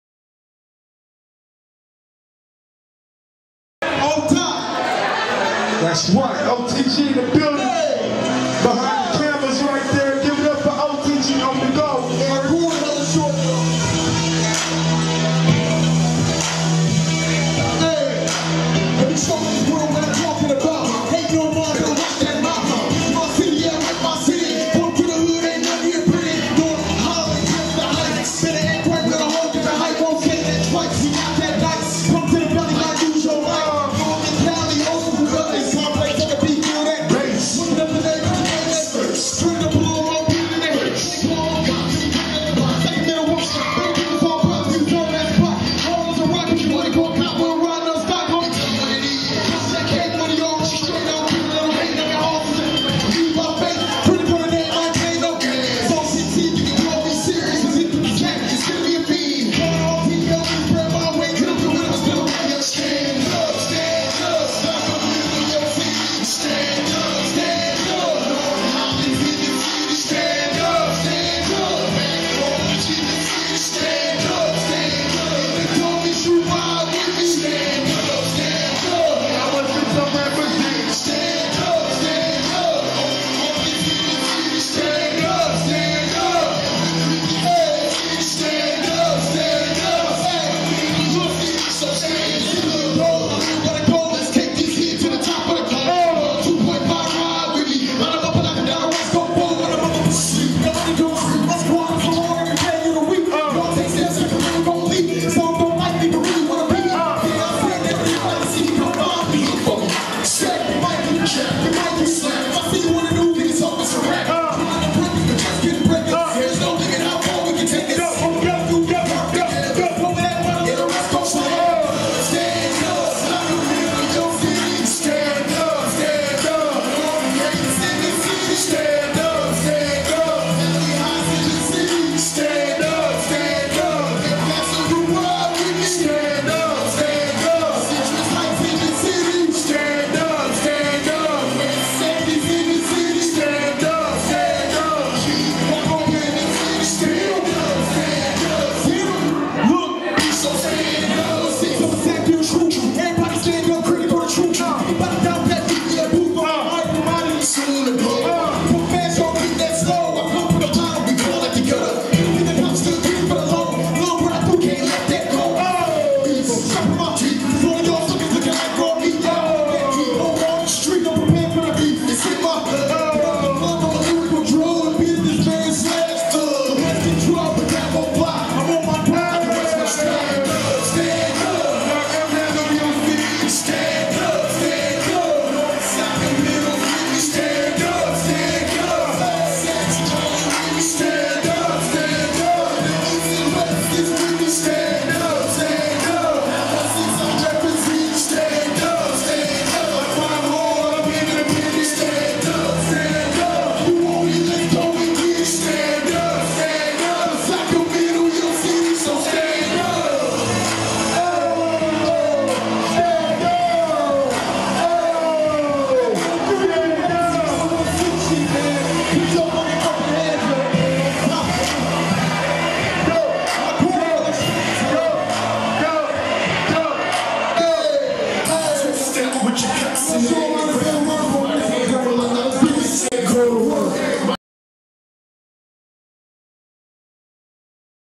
KRITICAL FROM SAC 916. STAND UP
KRITICAL FROM SAC 916 STAND UP. PUT UP ON YOUTUBE BY ON THE GO PRODUCITONS.  PERFORMED AT THE MAIN STREET BREWERY IN ROOSEVILLE. OTGDVDS